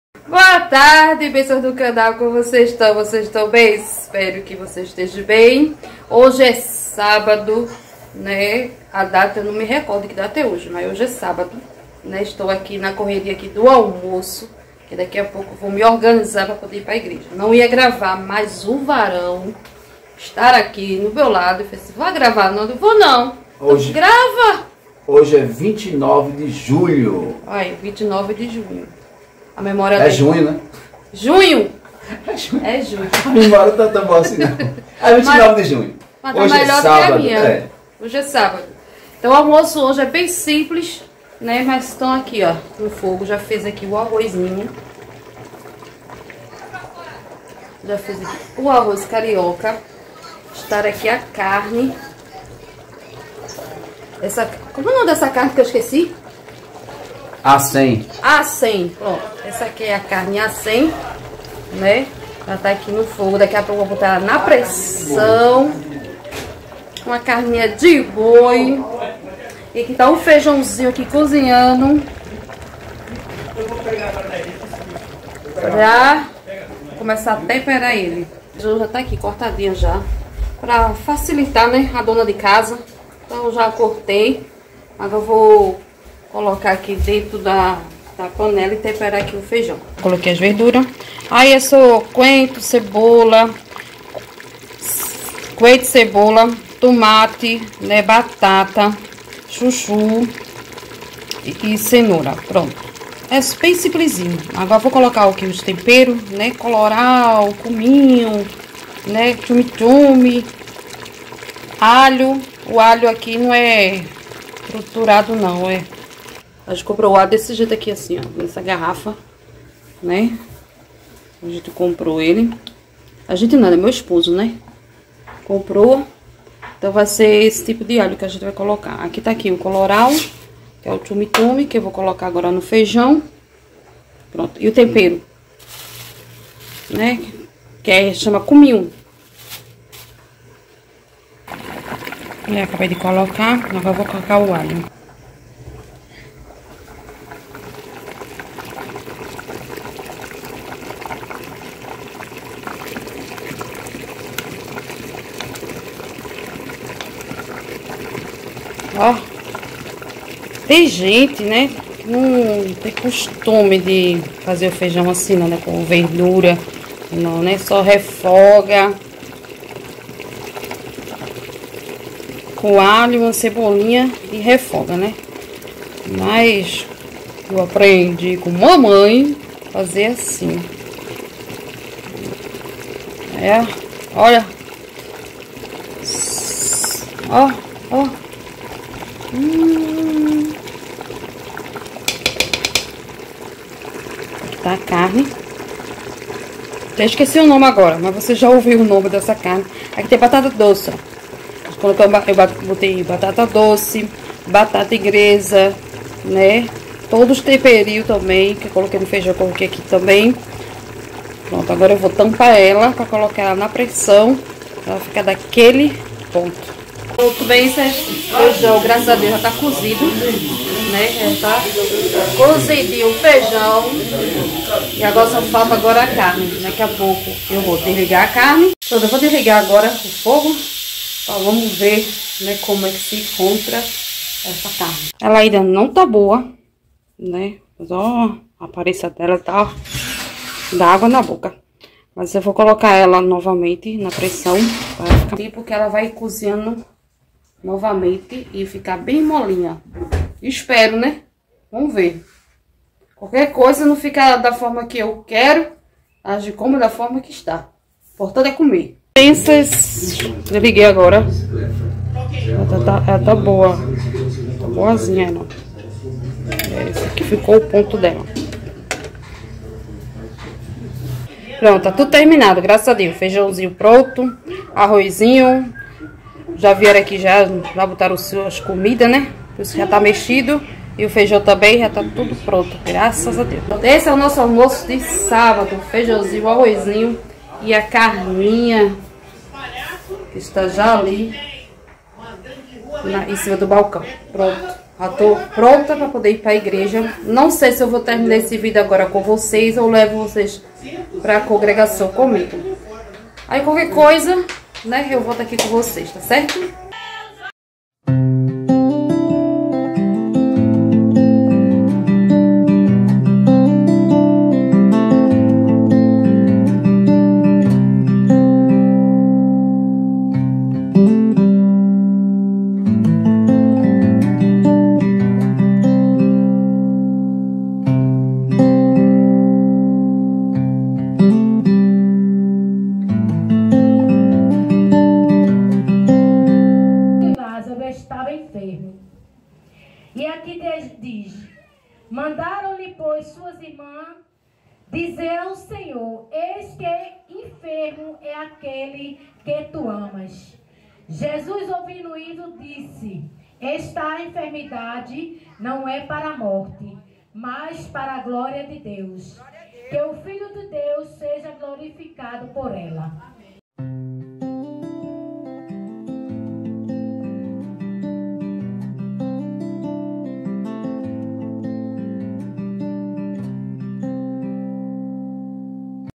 a preparação daquele almoço simples & me preparando para ir à igreja
olá bença seja bem-vindo neste vídeo vocês vão ver um pouco do meu dia  a preparação do almoço simples e  um momento de adoração na casa de Deus . 😊😊